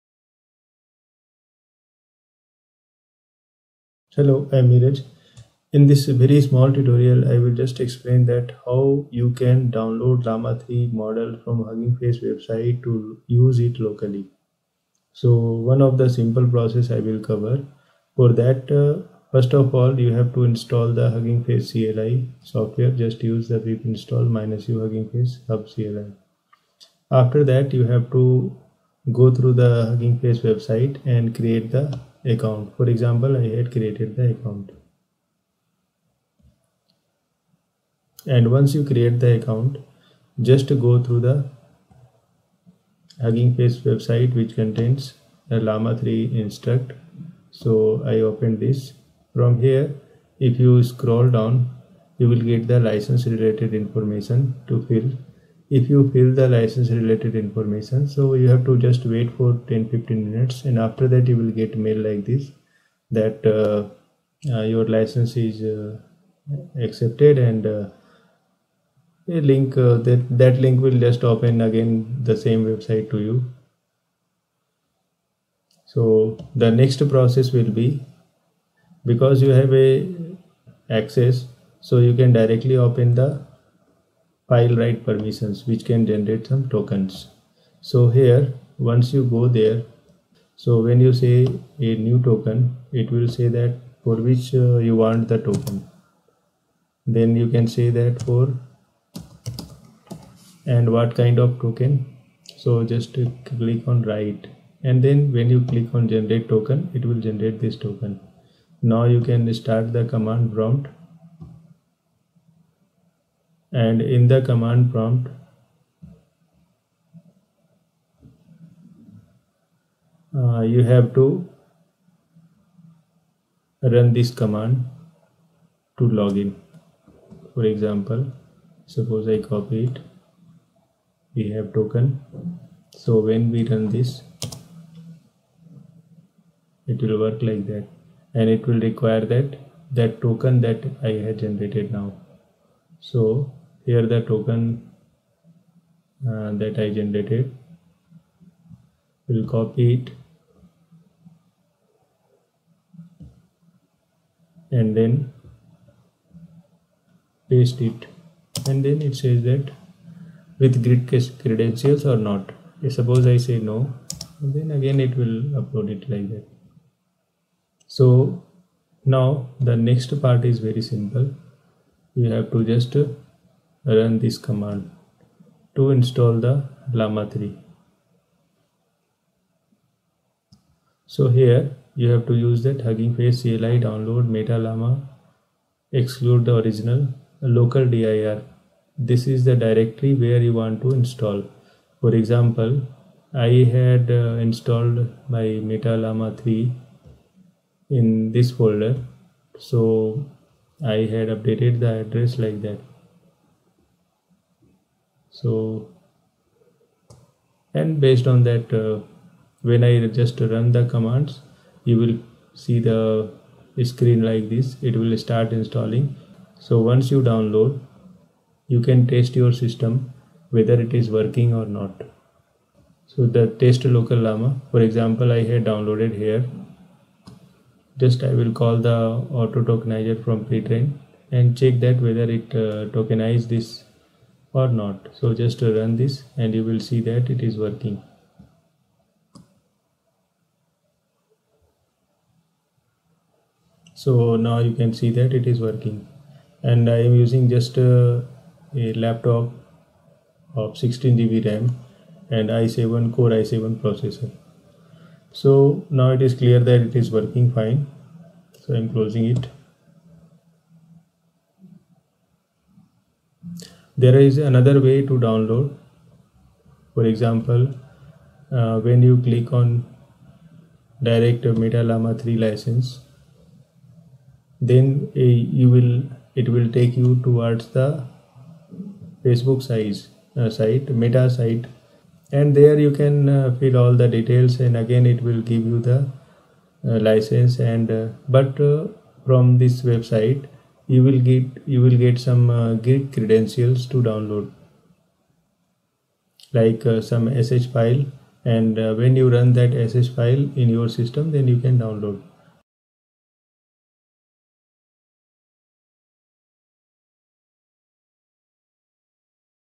Download and Use Llama-3 Locally
Contains.
A few easiest process (other than using Llama-3 through Ollama )
Code-Demonstration

Steps to download Meta-Llama3:
1. Install Hugging Face CLI:
pip install -U "huggingface_hub[cli]"

2. Create a Hugging Face account if you don’t have one

3. Accept the model’s conditions and privacy policy for Llama-3–8B-Instruct Wait until you are granted access. Usually takes around 15 minutes, but it can take more time.

4. Generate token (with write permission)

5. Login to Hugging face through command prompt. Log in to your Hugging Face account:
huggingface-cli login

6. Download the model (specify the path you want):
huggingface-cli download meta-llama/Meta-Llama-3-8B-Instruct --exclude "original/*" --local-dir D:\\OLLAMA_MODELS\\Meta-Llama-3-8B-Instruct